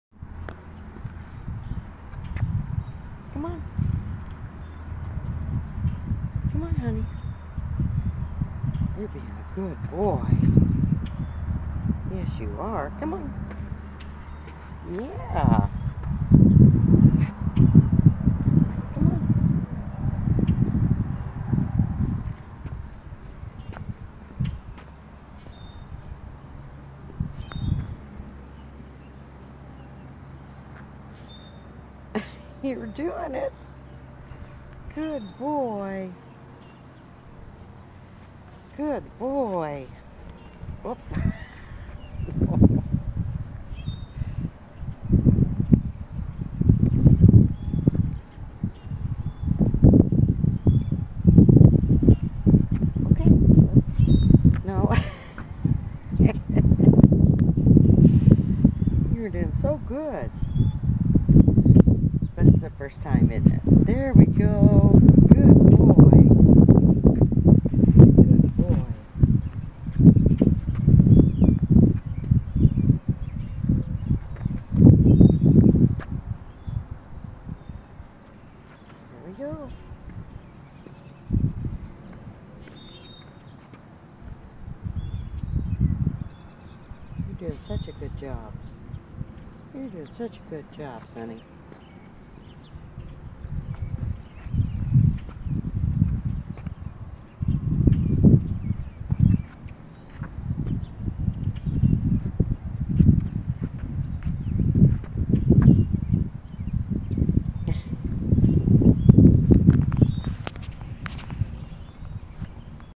Sunny's First Cat Walk
I'd tried to "walk" previous kitties to no avail; they would always just lay down and fight the harness.  But I fervently hoped Sunny just might take to it.  A neighbor graciously gave me the harness/leash as her kitty "hated it."  We are both thankful to her for her kindness. Sunny is a male, but even so, I think he looks pretty snazzy in pink.  Let the adventures begin!

* I apologize for the loud wind noises at times.